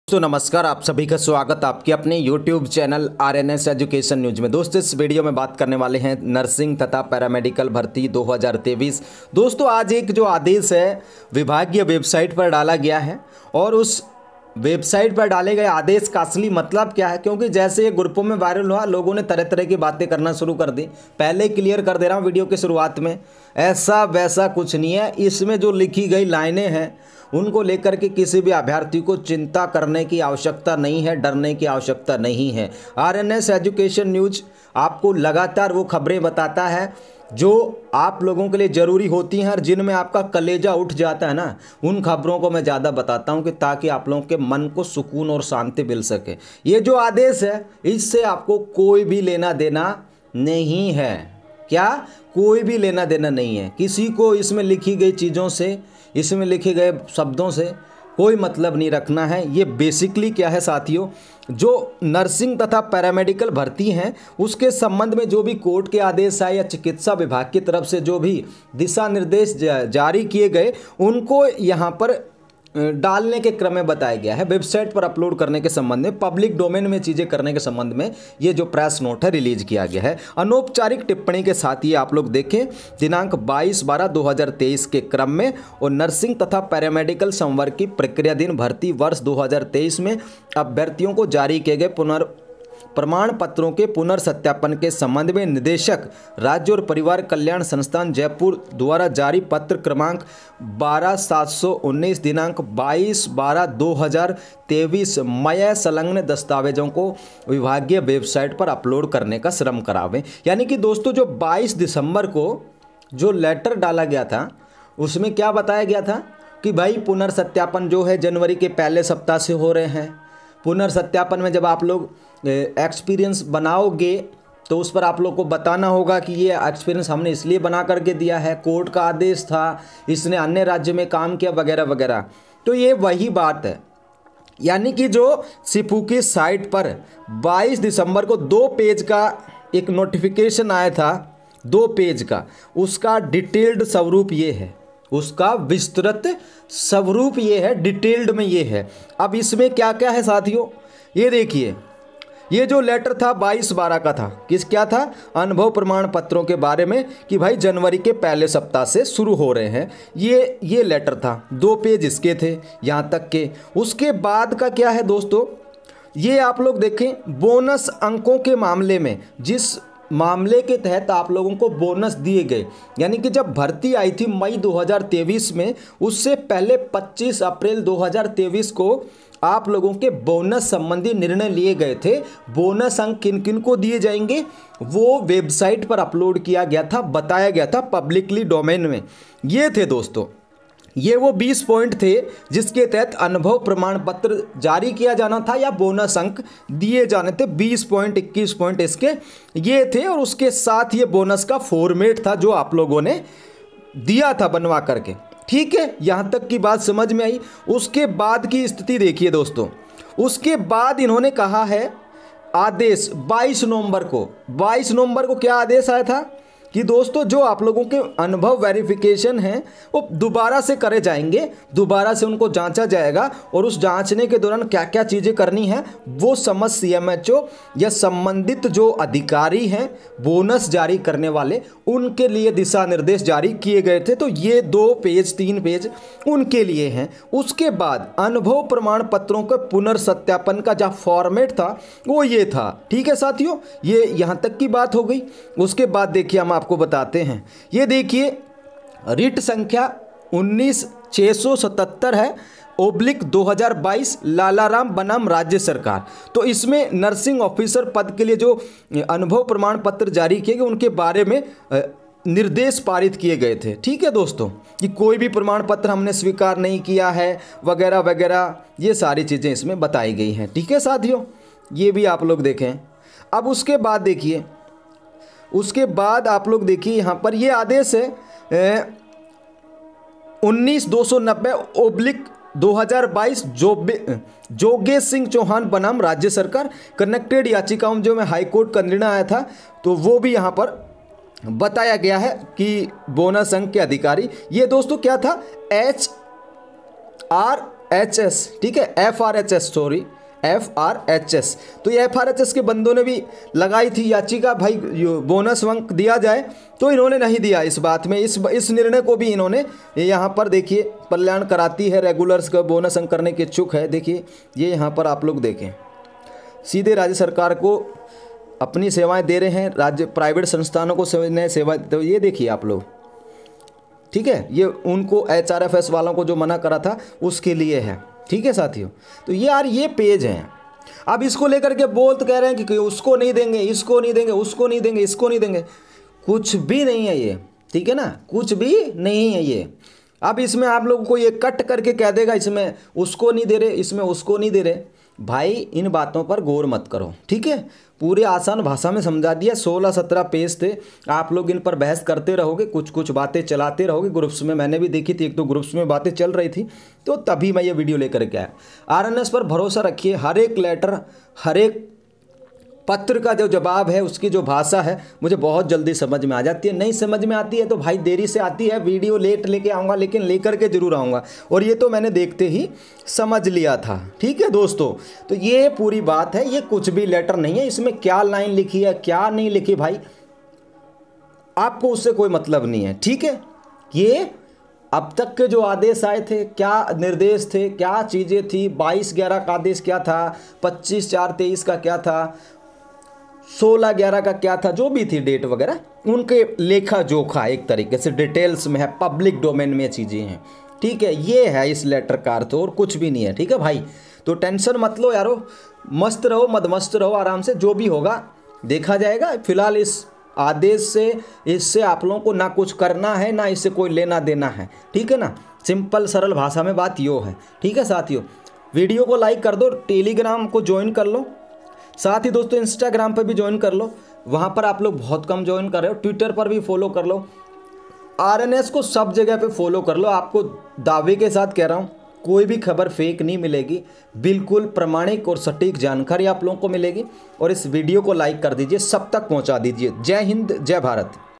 नर्सिंग तथा पैरामेडिकल भर्ती /एक और आदेश जारी /विभाग ने जारी किया आदेश -देख लो क्या है पूरी खबर
RNS Education News -हमारे सोशल मीडिया प्लेटफार्म 👇
ऑफिसियल वेबसाइट लिंक 👇

ट्वीटर 👇 Twitter